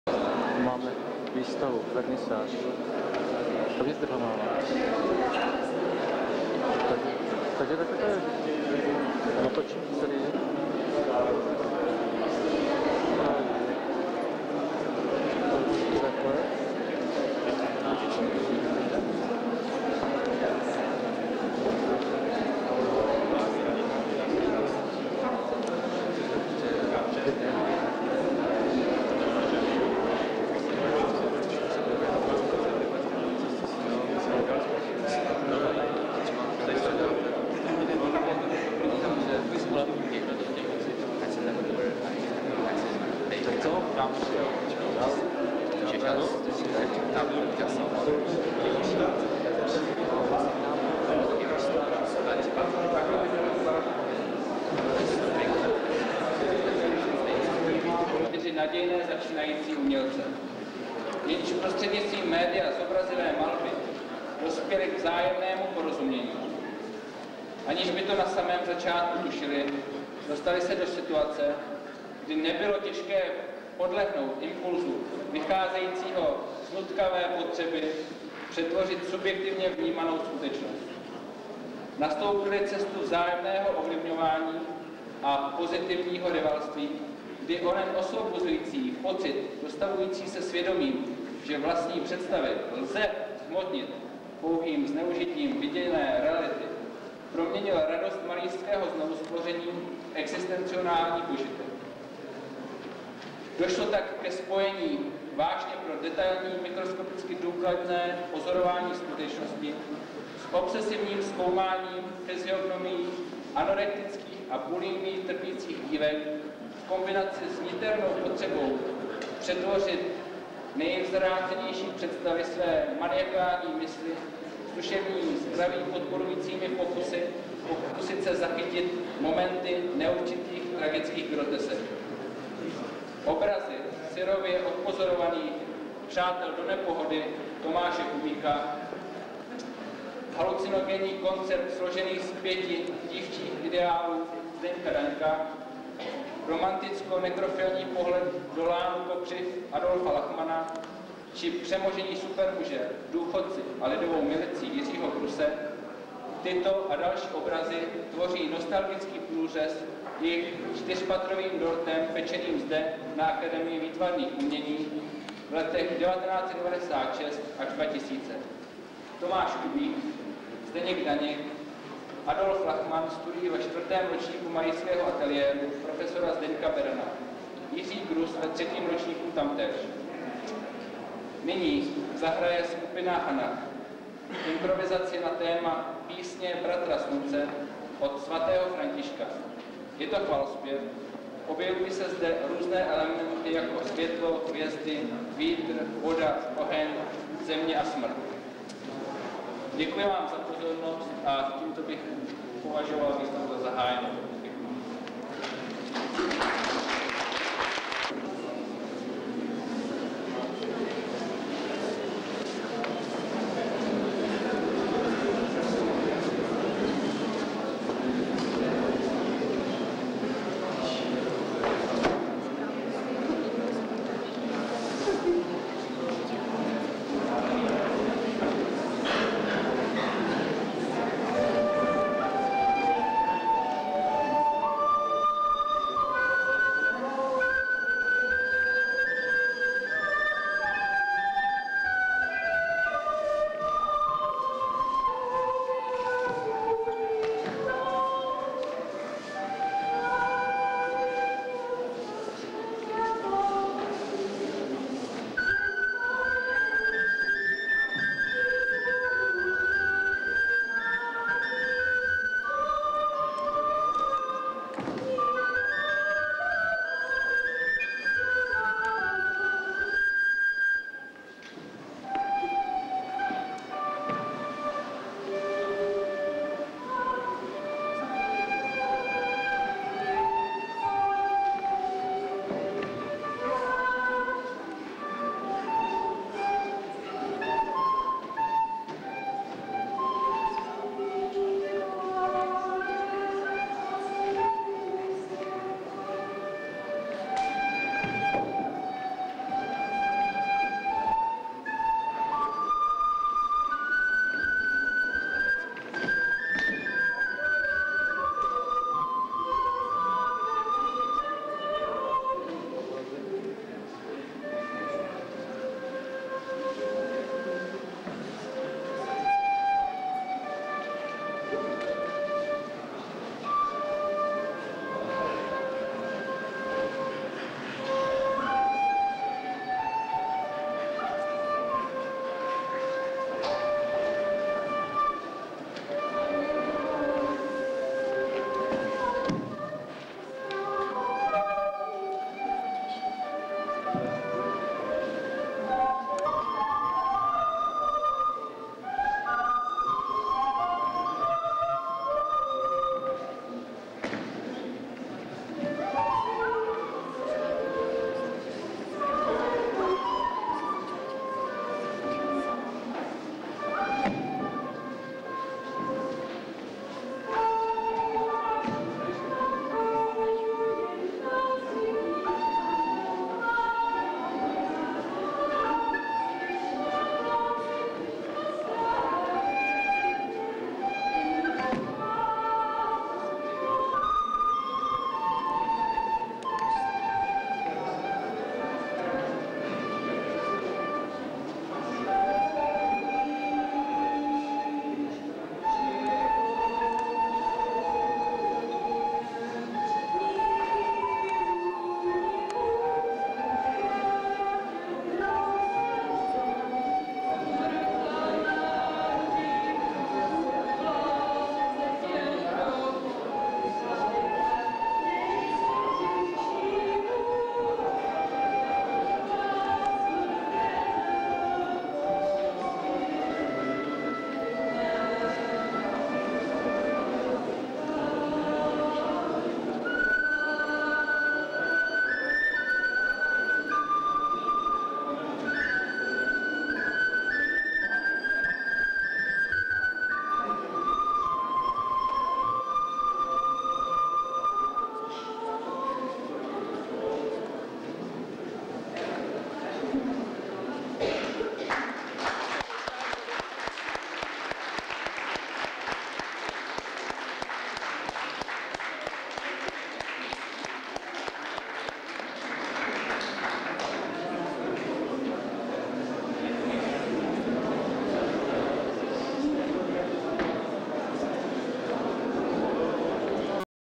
Kamaradi (2000) cast1
Vystava Kamaradi v galerii AVU, Praha, 2000,
vystavujici: Danek, Grus, Kubik, Lachman